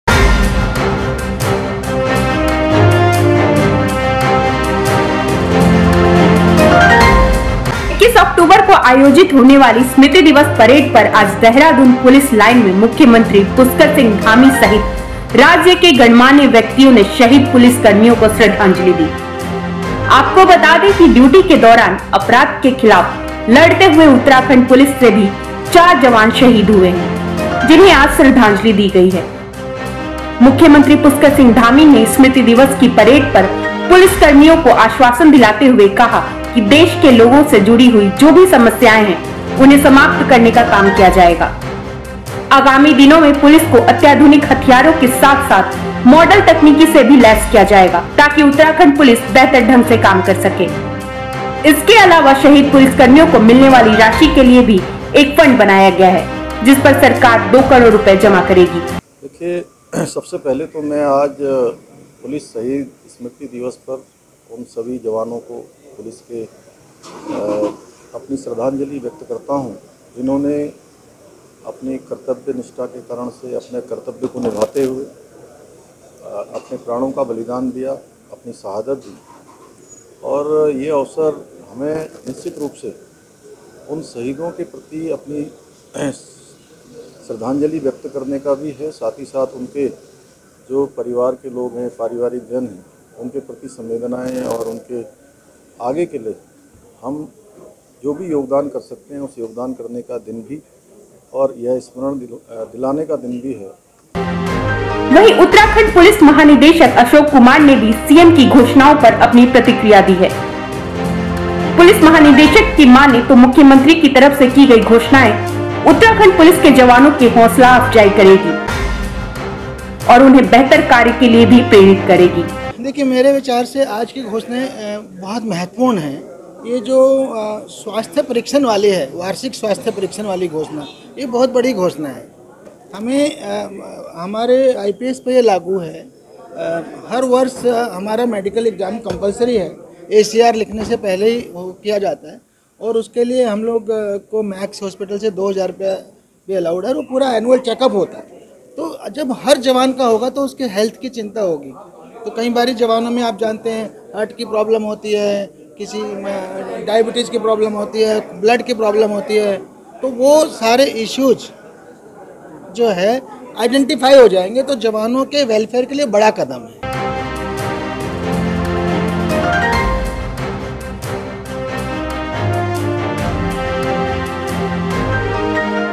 स्मृति दिवस पर CM धामी ने शहीद पुलिसकर्मियों को दी श्रद्धांजलि ||CMDHAMI
21 अक्टूबर को आयोजित होने वाली स्मृति दिवस परेड पर आज देहरादून पुलिस लाइन में मुख्यमंत्री पुष्कर सिंह धामी सहित राज्य के गणमान्य व्यक्तियों ने शहीद पुलिसकर्मियों को श्रद्धांजलि  दी।